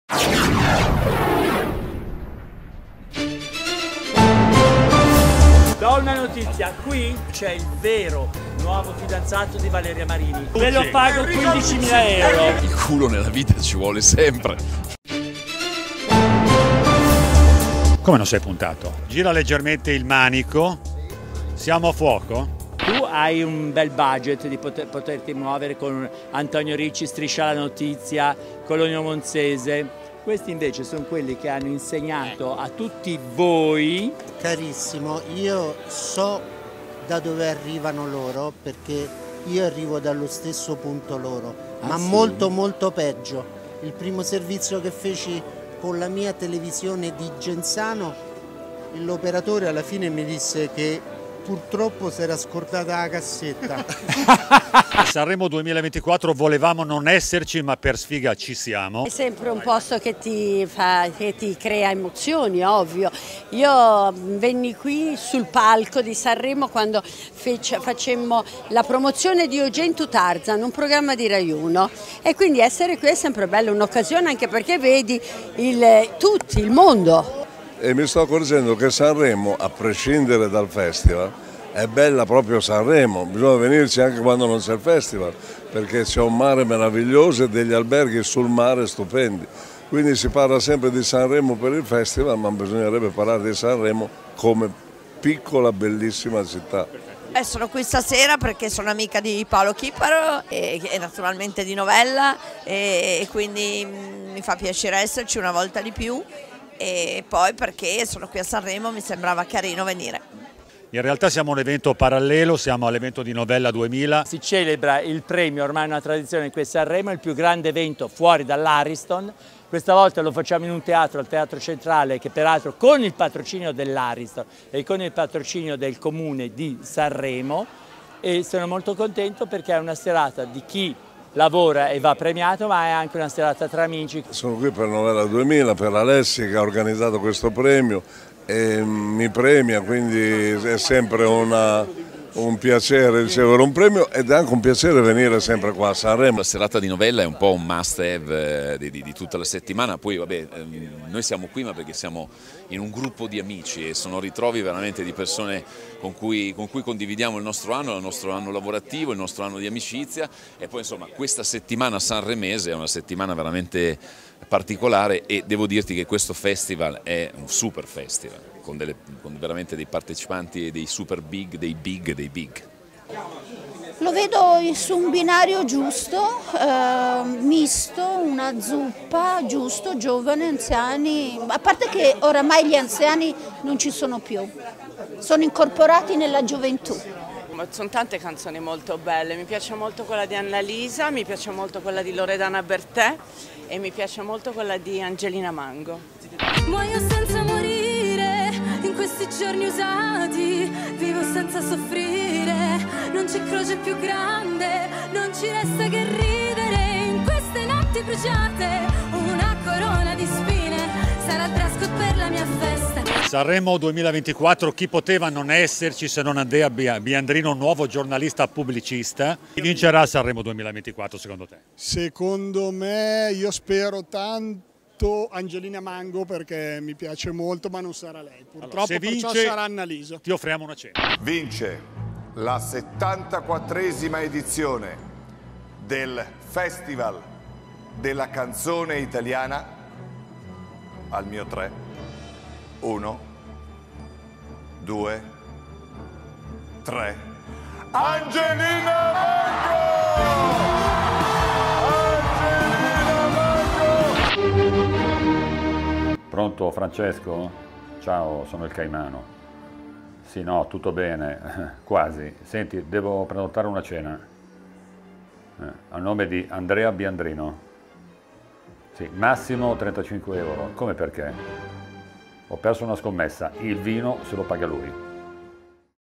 Occhio al Caimano: Festa Novella 2000 a Sanremo
Il Caimano ha incontrato i protagonisti della Festa di Novella 2000 a Sanremo